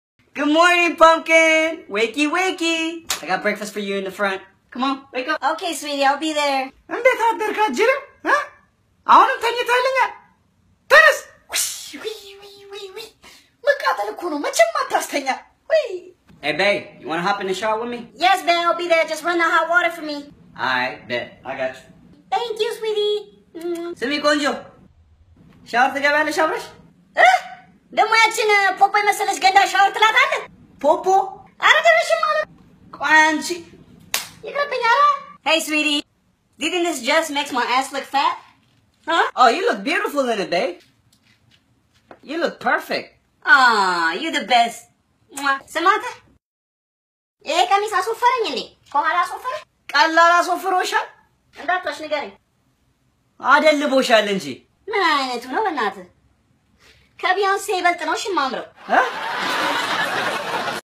Funny Habesha Videos ( Americans Vs Habeshas)
All the shirts you see in this video are available on there. Thank you for the support.

Comparing life in two different cultures in comedy way.